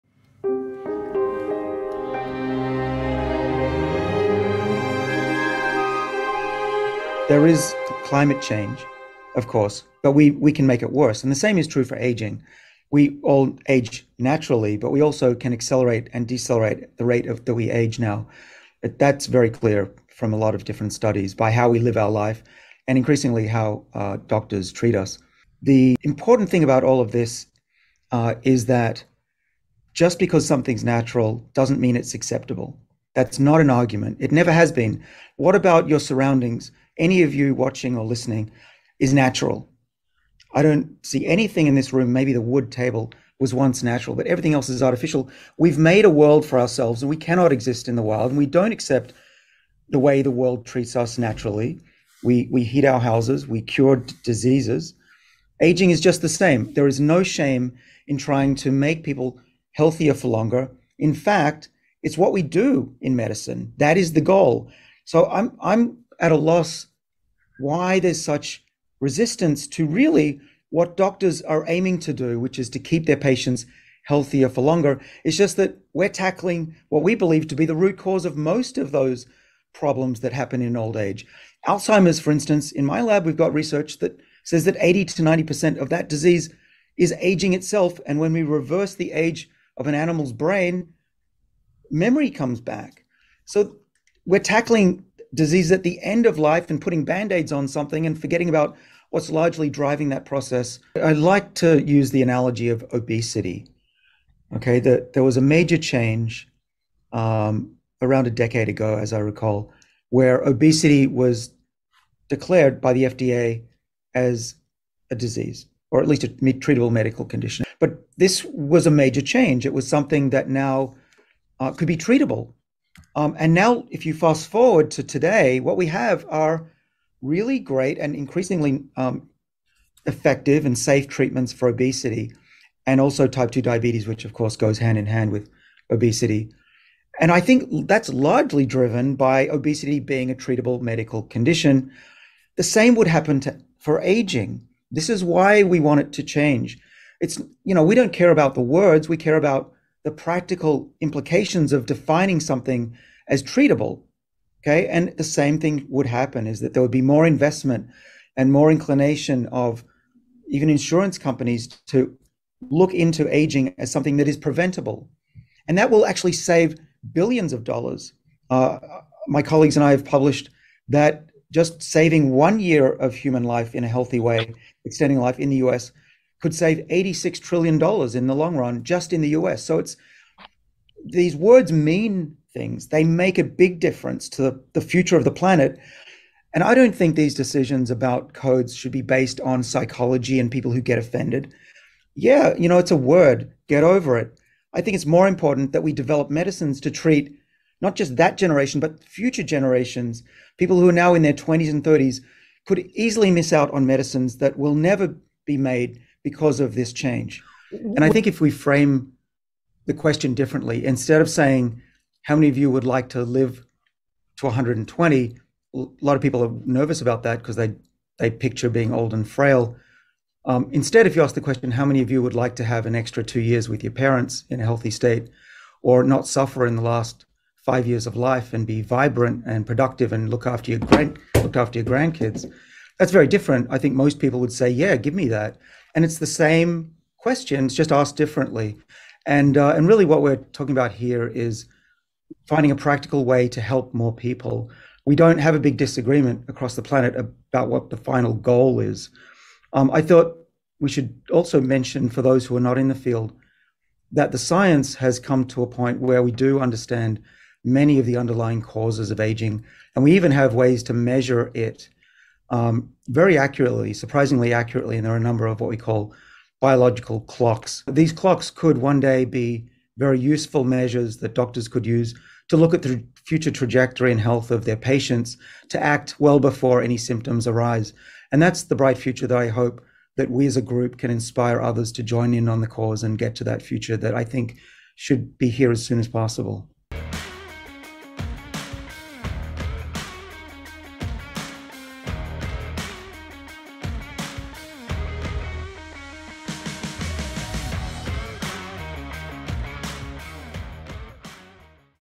DO WE NEED TO AGE? | Dr David Sinclair Interview Clips
Dr David Sinclair talks about why we don’t need to accept aging and his wish to help people to live healthier for longer in this short clip.

 iHerb All Supplements & Health Products Discount Link